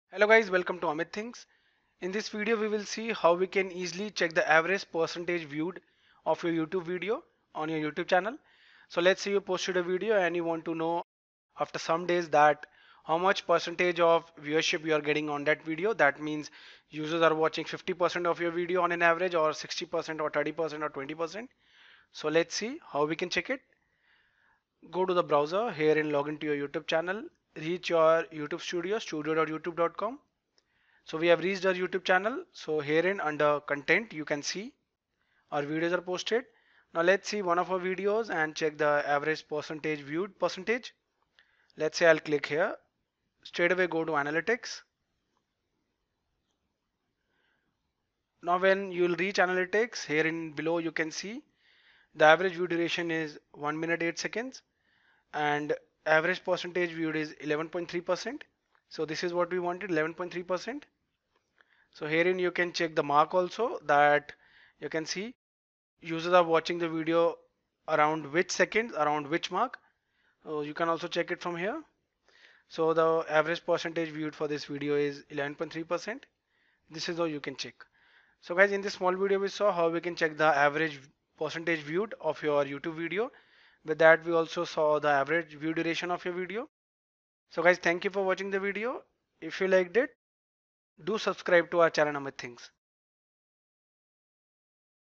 How to check Average percentage viewed of your YouTube video (2021)
In this video, learn how to check Average percentage viewed of your YouTube video.